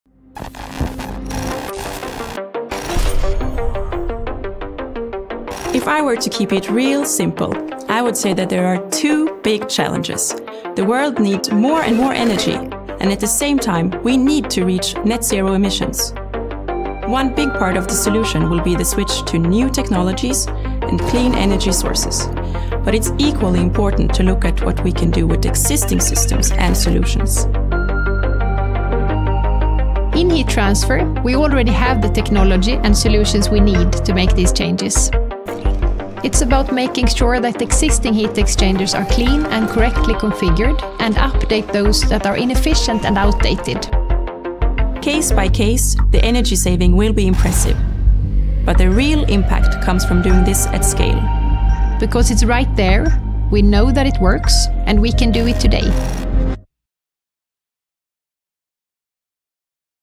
Energy Hunter by Alfa Laval
Energy efficiency is in our DNA. It is something we have been developing for almost a century. In fact, Alfa Laval's heat transfer solutions save 100 GW of energy every year. That is enough power to heat 20 million homes.